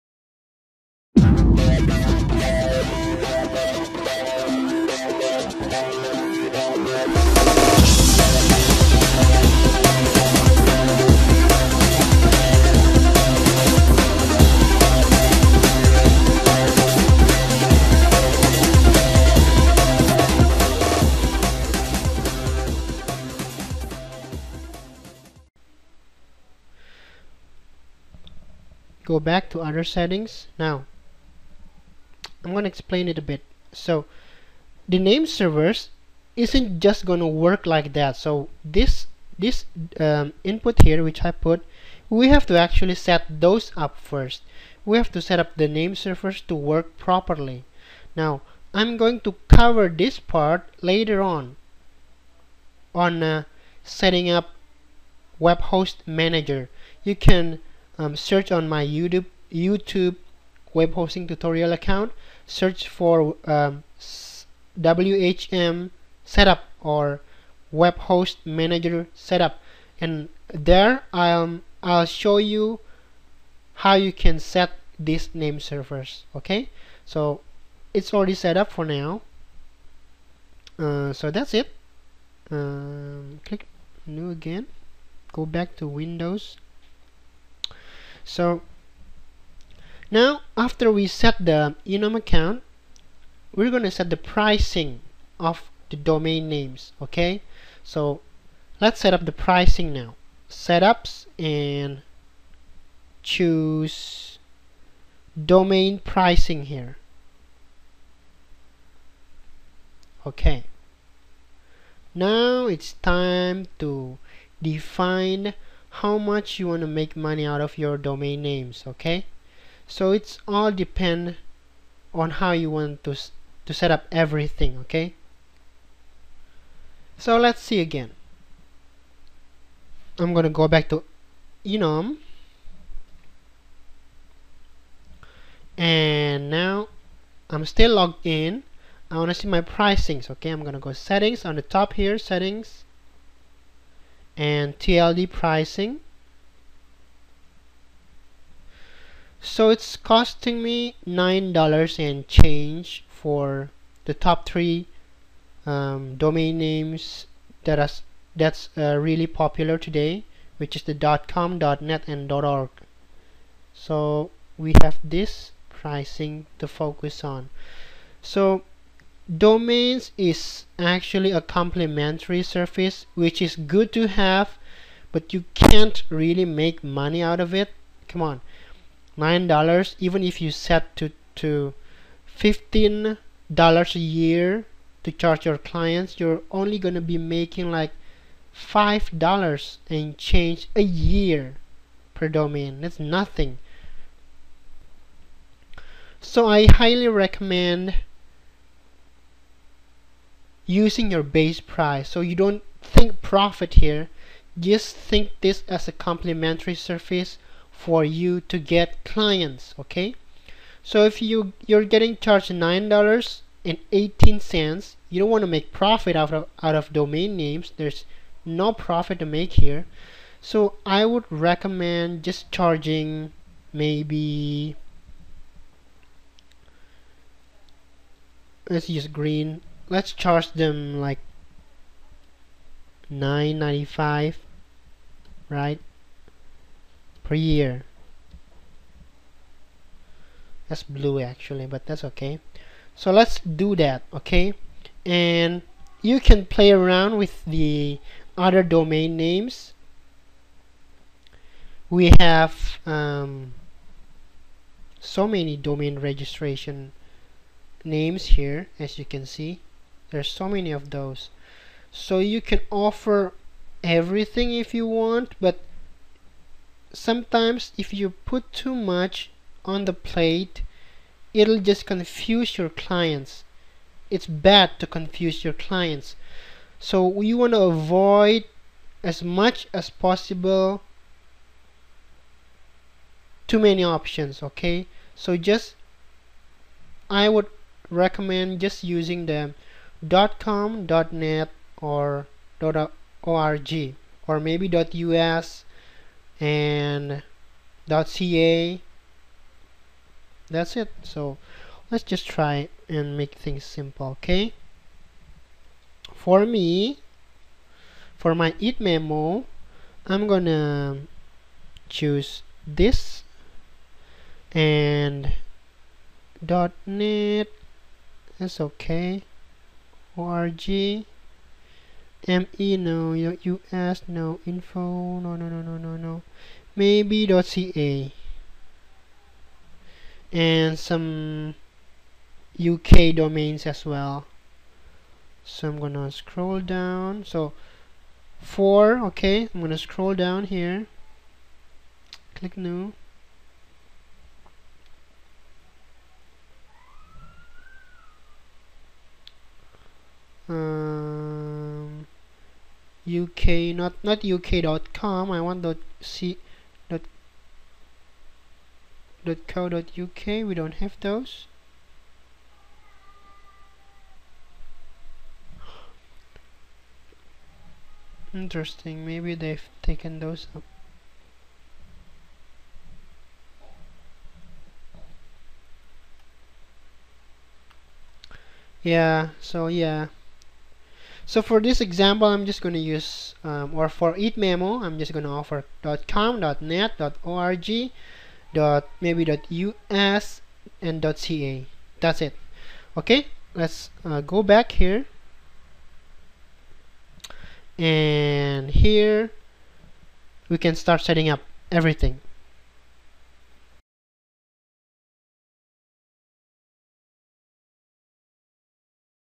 WHMCS Part 4 - Domain Names Setup (3/5)
Setting up domain registration service in WHMCS using enom